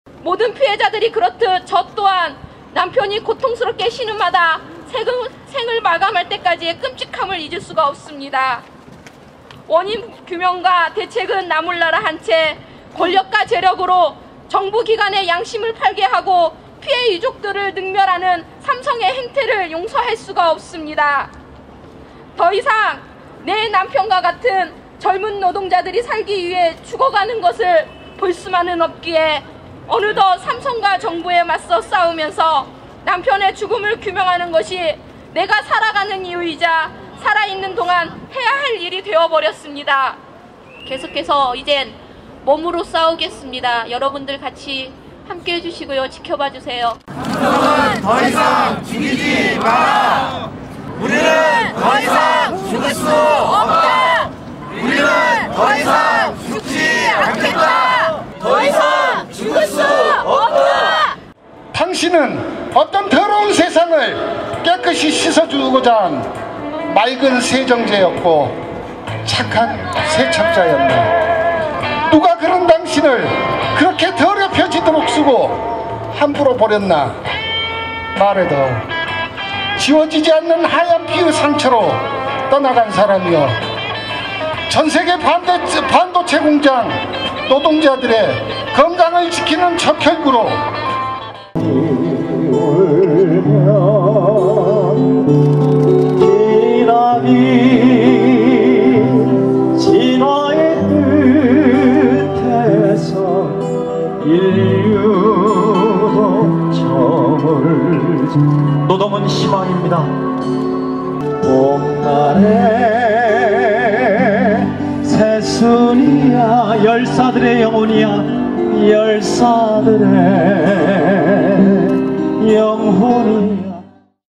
삼성에서 사망한 노동자 46명
반도체노동자의건강과인권지킴이, '반올림'은 6일 오후 서울역 앞에서 추모문화제를 열고 지금 까지 삼성 반도체·전자산업에서 일하다 사망한 노동자들의 산재인정을 촉구했다.